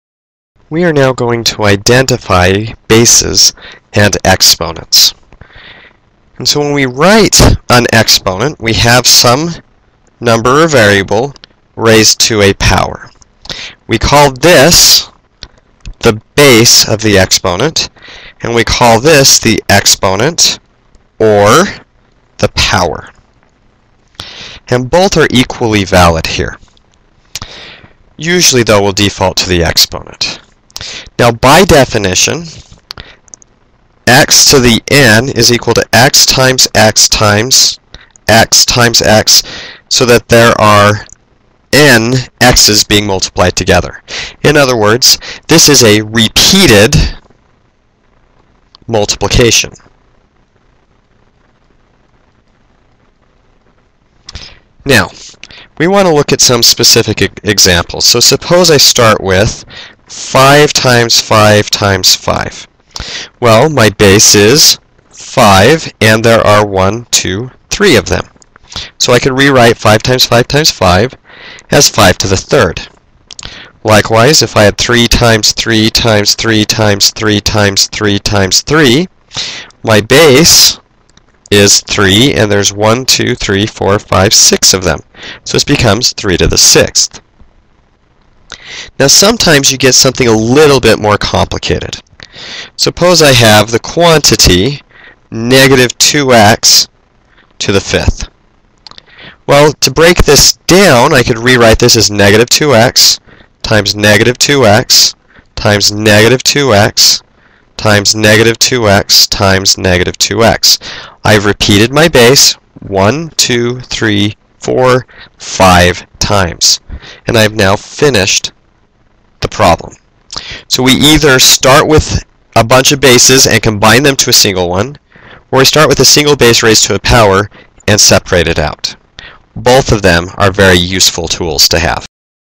5 1 1 Identify Bases and Exponents
Gives a brief description of exponential form and how to convert between it and the more general form.